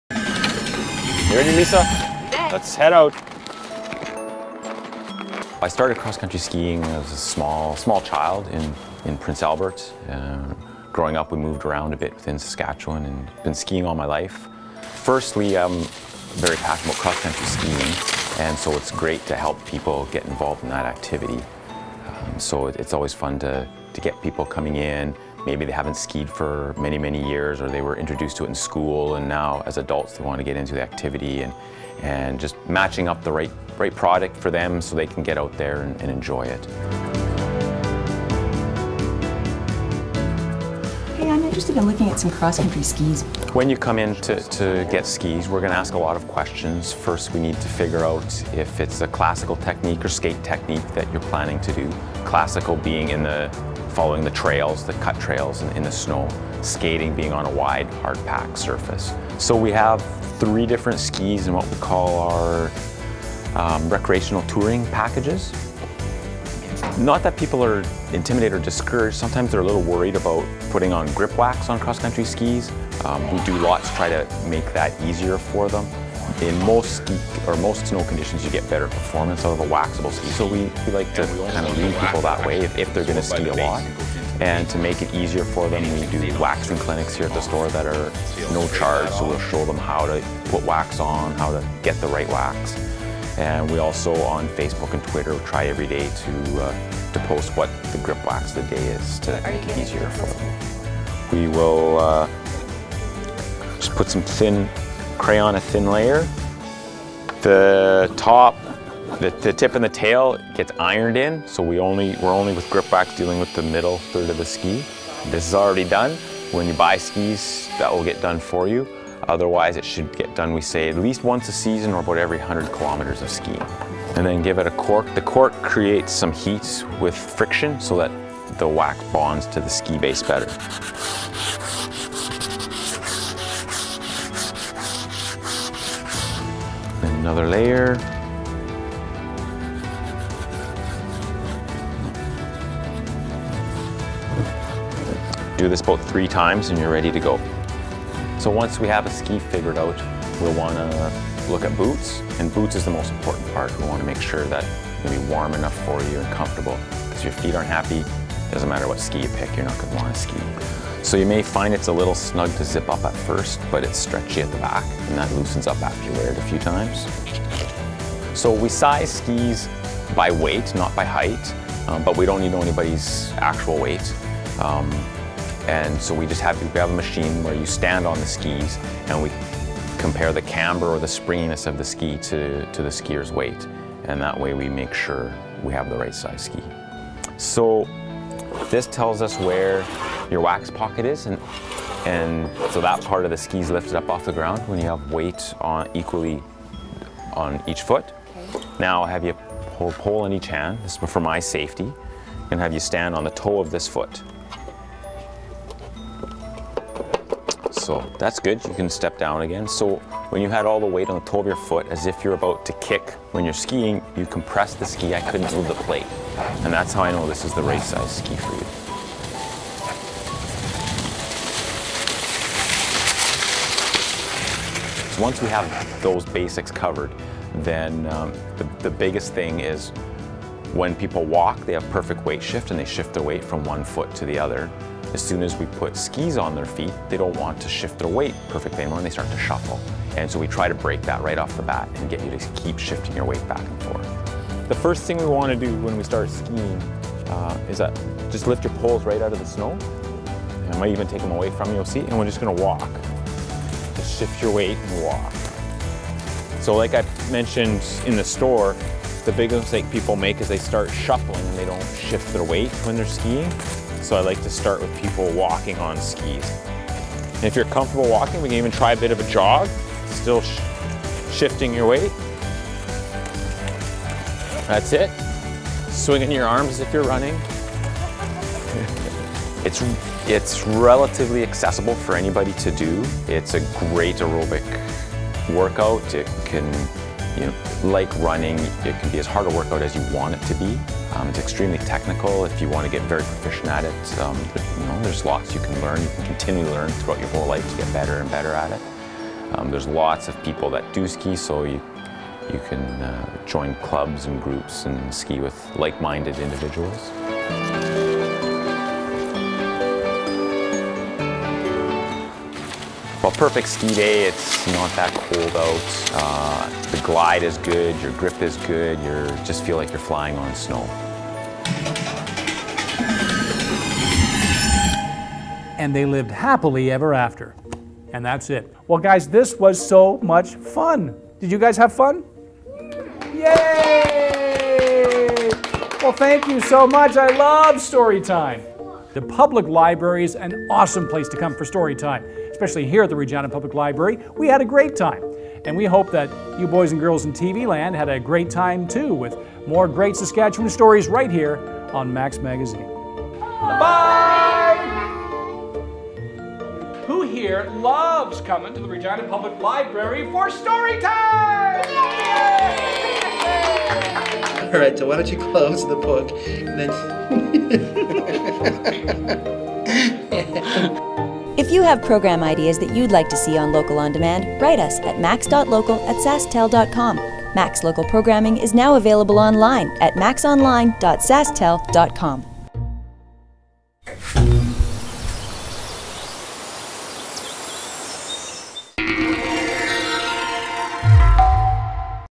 Kevin Robinson of Ebs Source For Adventure talks XC Skiing
One of the owners of Eb's, Kevin Robinson talks about the benefits and realities of cross country skiing.  He walks you through the process of choosing and buying a set of skis and gives some tips for beginners (or those re-visiting skiing after a long hiatus) as they venture out onto the snow.